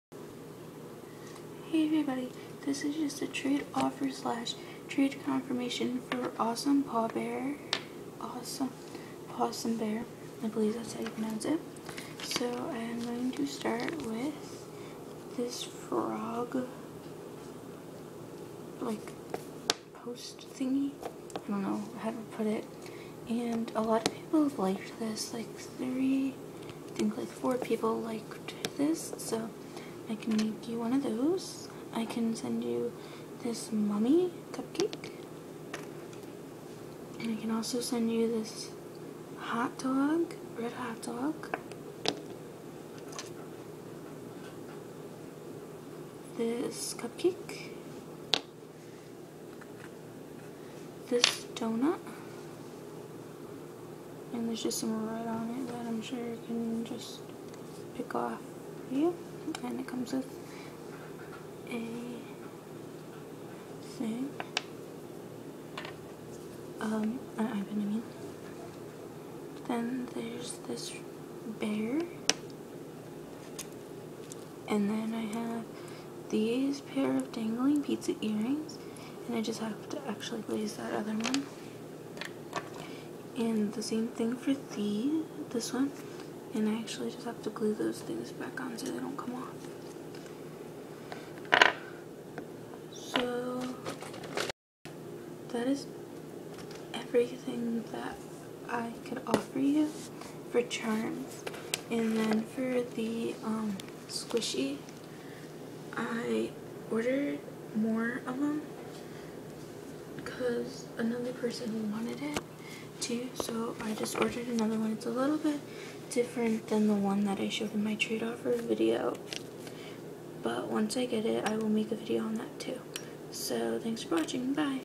Trade offer for awesomepawsomebear
The squishy will be in sometime this week if you want me to make one just tell me what and I'll make it hope we can trade